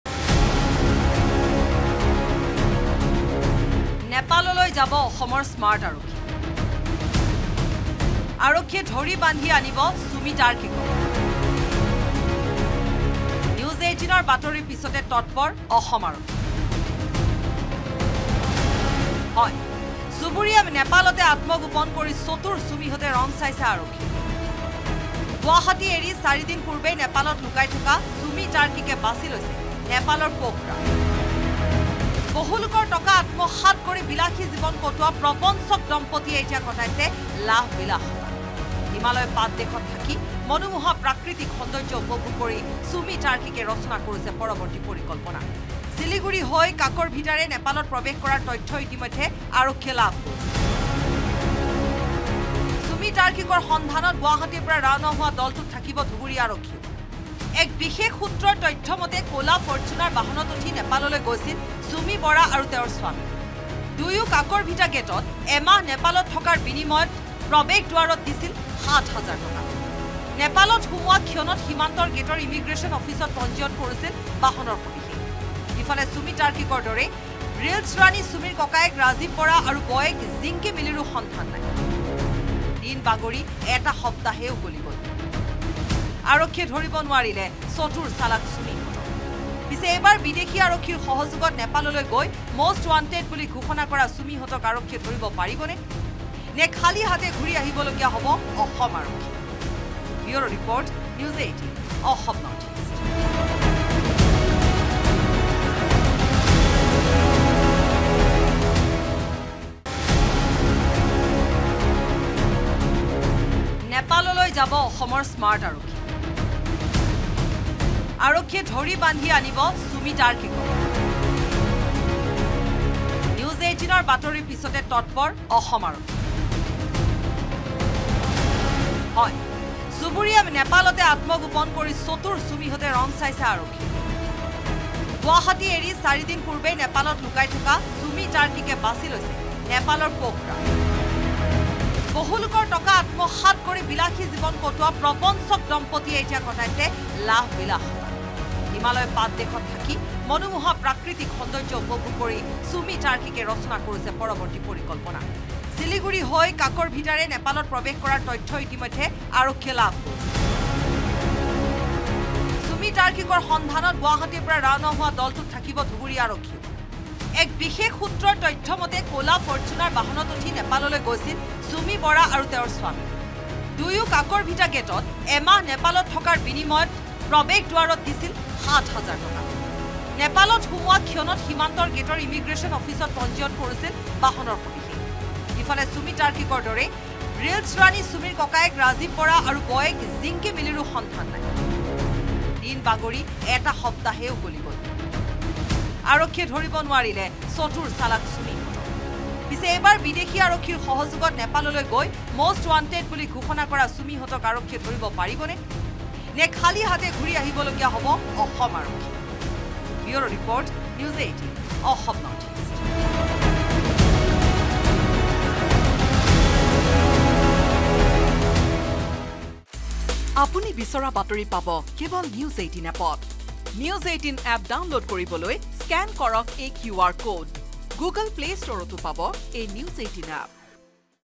Sumi Borah | নেপালত আত্মগোপন চুমি-তাৰ্কিকৰ | Assam Trading Scam | N18V
নেপালত আত্মগোপন চুমি-তাৰ্কিকৰ
কাকৰভিটালৈ ৰাওনা অসম আৰক্ষীৰ দল
কাকৰভিটাৰ গেটতে আছে তথ্য প্ৰমাণ
ক’লা ৰঙৰ ফৰ্চুনাৰসহ ২ ঠগ আছে পোখৰাত
NEWS18ৰ বাতৰিত আৰক্ষীৰ হাহাকাৰ